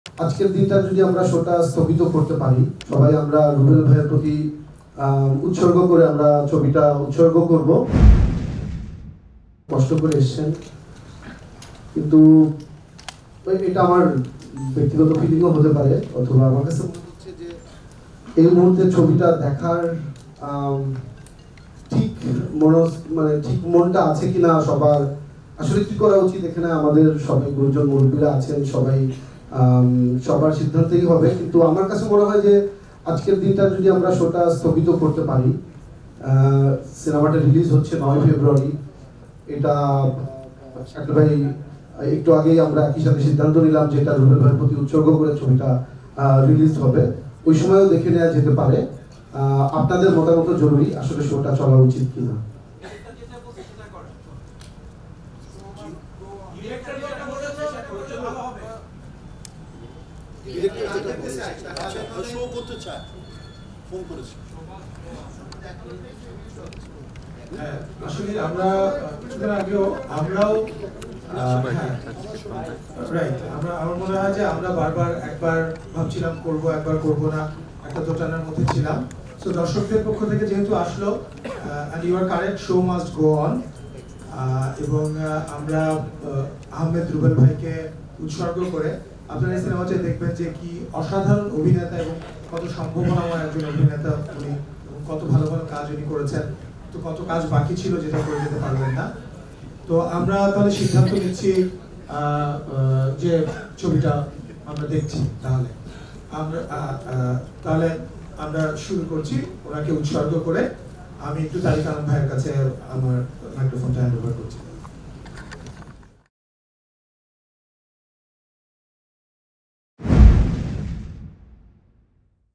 ছবিটি আহমেদ রুবেলকে উৎসর্গ করলেন টিম 'পেয়ারার সুবাস' | Ahmed Rubel | Peyarar Shubash | Chitralee
৭ ফেব্রুয়ারি জানা গেলো 'পেয়ারা সুবাস’-সিনেমার অন্যতম মূল চরিত্রের অভিনেতা আহমেদ রুবেল আর নেই। সিনেমার মধ্যে দিয়ে শিল্পী রুবেল-কে তারা শ্রদ্ধা নিবেদন করতে  'পেয়ারা সুবাস’ টিমের সবার মতামত সাপেক্ষেই অভিনেতা রুবেল-কে ‘পেয়ারার সুবাস’ সিনেমাটি উৎসর্গ করার ঘোষণা দেন নির্মাতা রেদওয়ান রনি।

Editor, Chitralee: Syeda Farzana Zaman Rumpa
Chitralee Team: Rahnama Haque, Nufsat Nadrun
Video Editor: Mostafijur Rahman